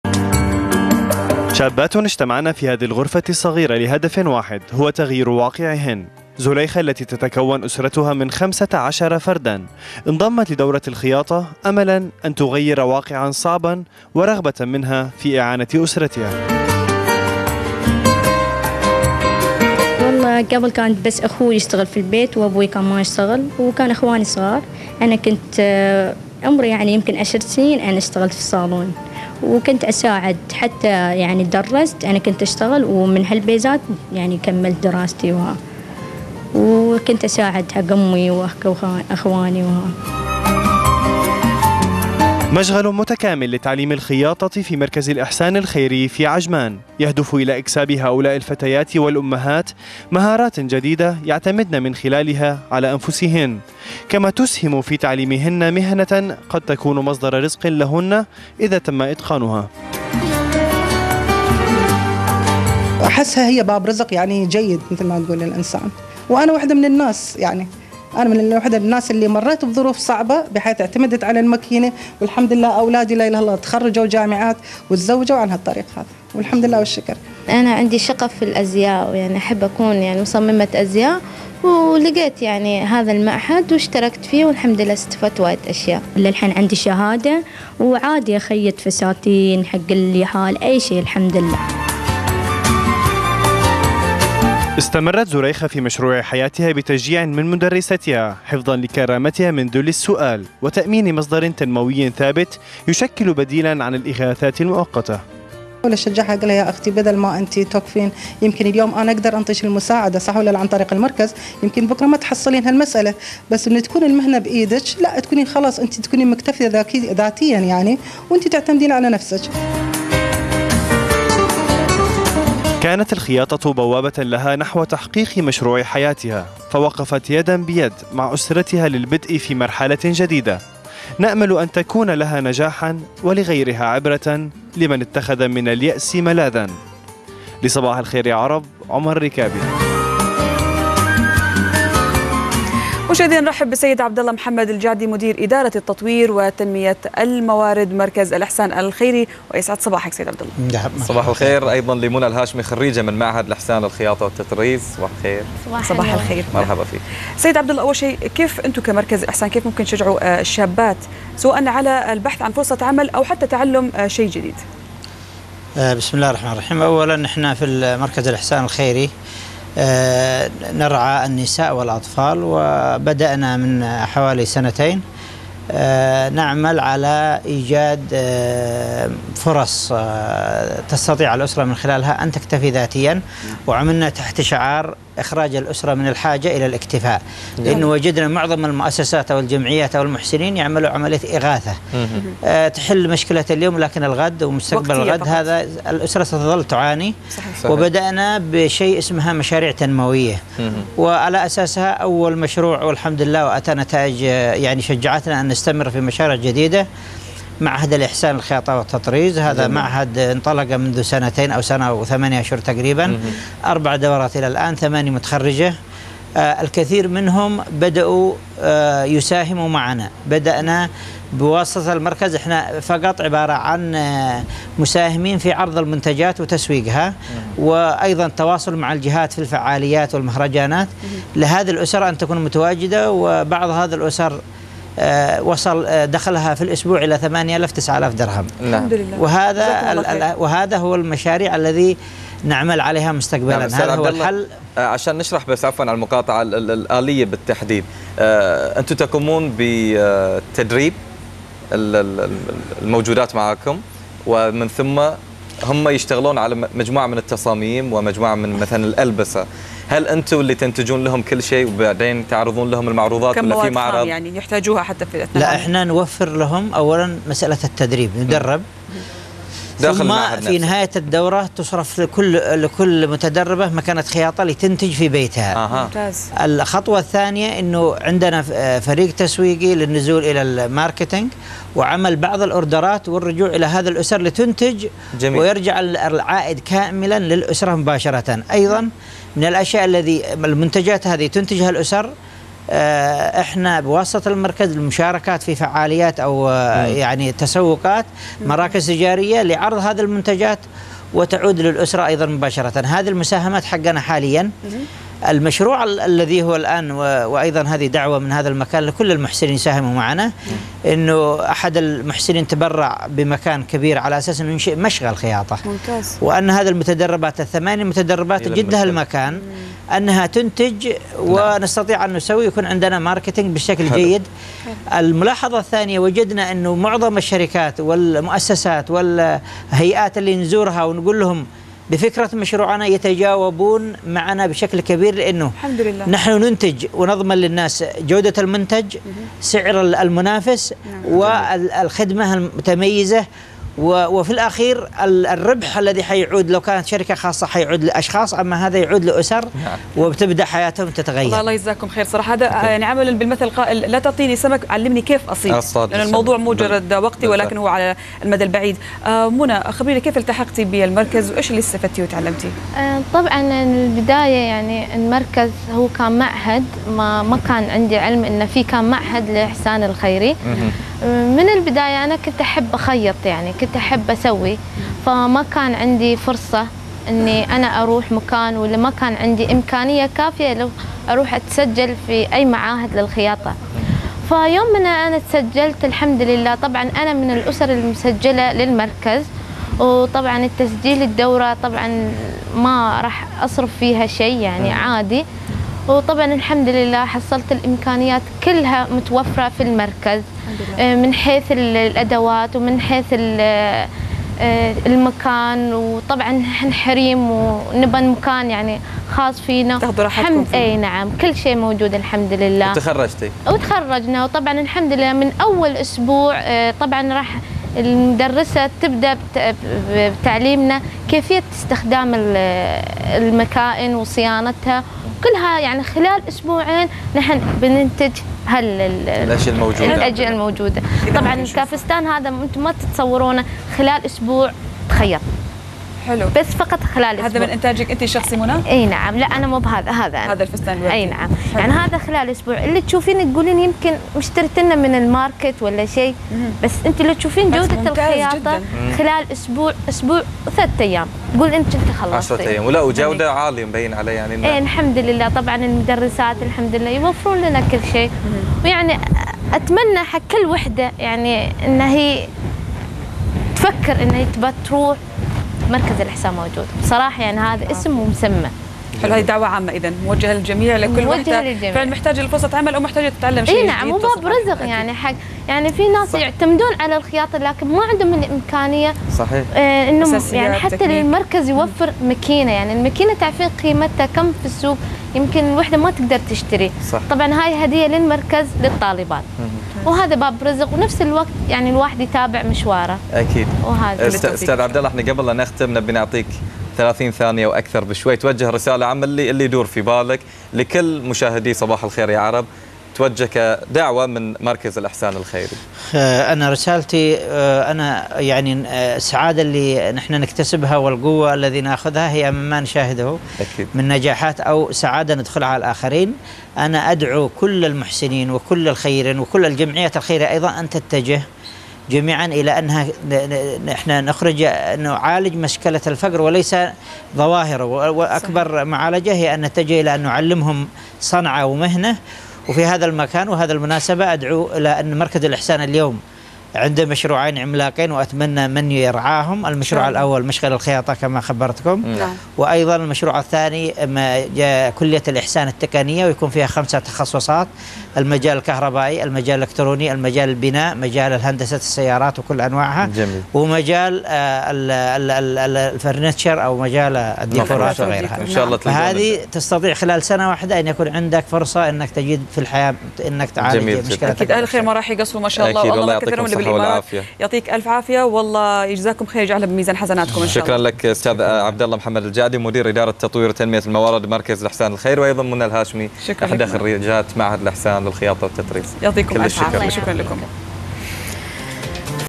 دورات خياطة مجانية للسيدات من ذوي الدخل المحدود
بهدف تمكين النساء وتأهيلهم يقدم مركز الاحسان الخيري لتعليم الخياطة والتطريز في عجمان يقدم دوراتها المجانية للشابات والامهات على مهارت الخياطة لتكون لهن ولاسرهن مصدر رزق